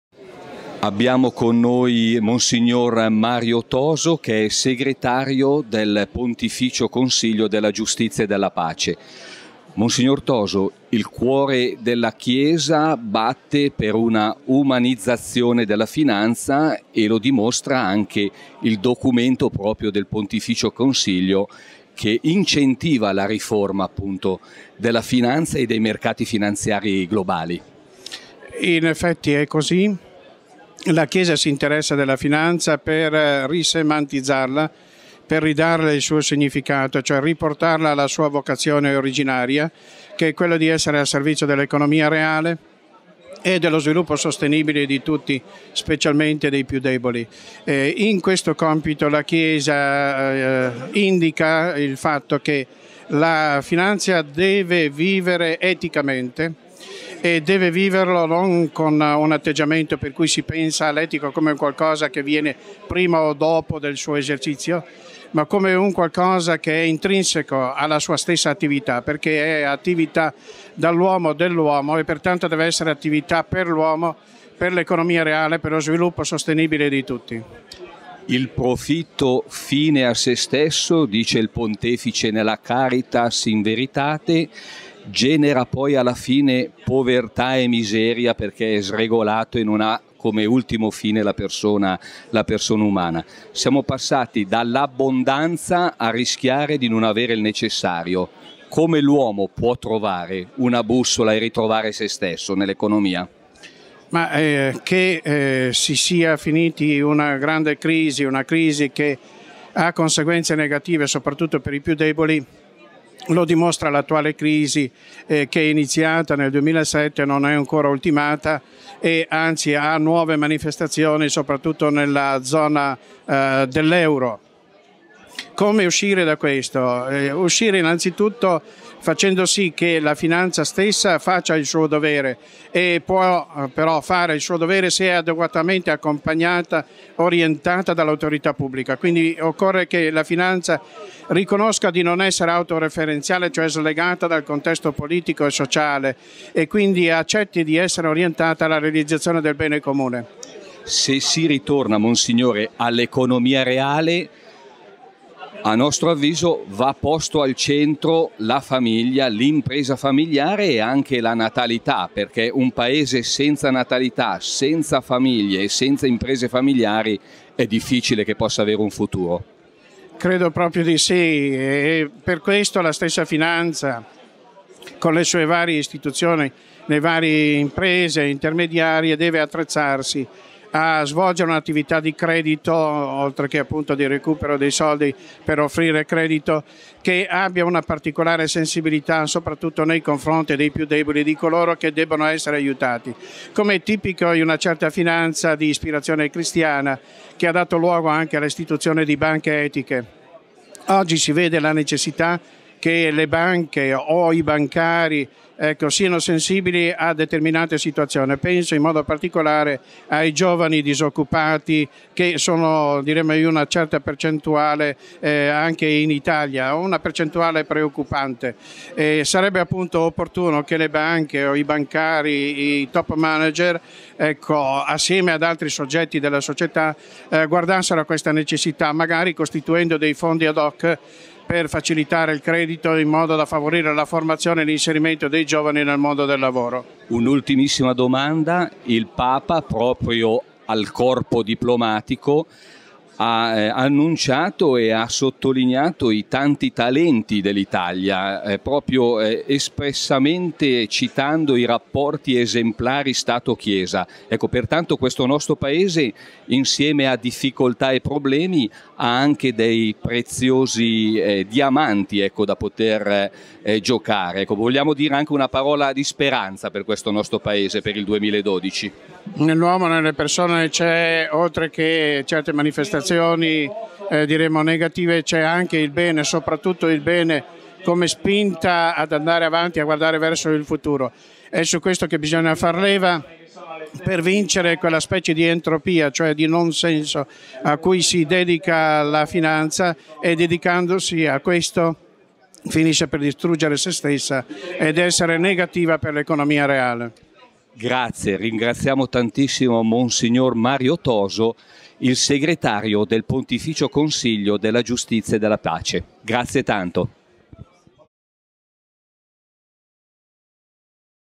Intervista Monsignor Mario Toso, Segr. Pontificio Cons. Giustizia e Pace
Intervista a Monsignor Mario Toso, Segretario del Pontificio Consiglio della Giustizia e della Pace, in occasione del convegno FIBA CISL "Per una Riforma del Sistema Finanziario" tenutosi a Roma il 24 gennaio 2012